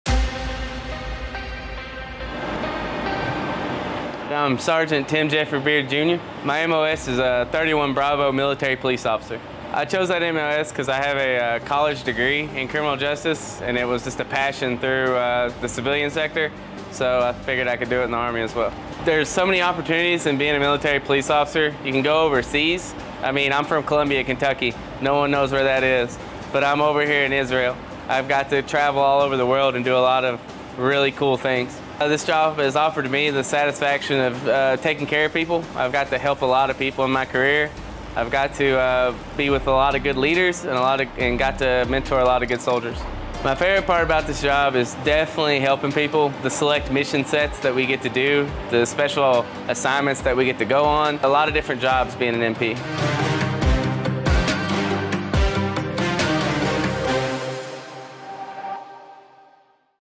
U.S. Army | 13B Military Police Officer Sgt. Tim J. Beard Jr.
U.S. Army Sgt. Tim J. Beard Jr., assigned to 13th Missile Defense Battery, shares his experience as a military police officer June 25 while deployed to Israel. 10AAMDC troops deployed to work consistently with Israeli Defense Forces to strengthen the interoperability necessary for maintaining and improving Israel's air and missile defense capacity.

13B Military Police Officer
ISRAEL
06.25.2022

Film Credits: Video by Pfc. Yesenia Cadavid
10th Army Air and Missile Defense Command